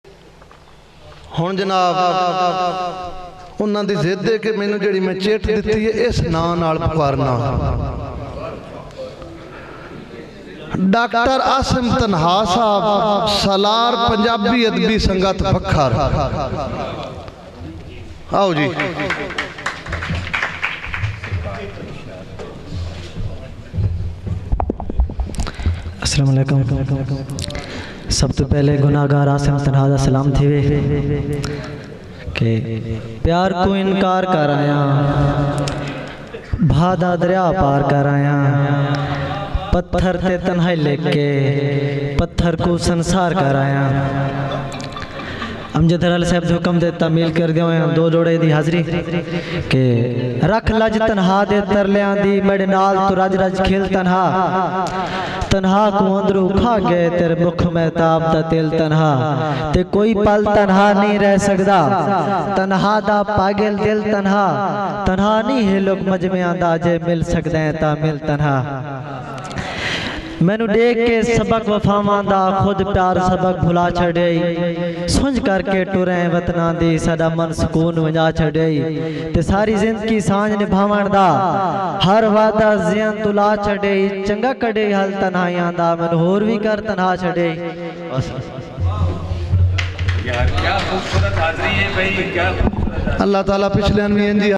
ASAM TANAHA
Punjabi Adabi Parchar (R) Islamabad pakistan di chattar chaven jagat maa boli dihar 21-2-16 ty Mehfil e Mushaira Pakistan Acadmy of Letters Islamabad .uploaded by Mehmood Amjad Haral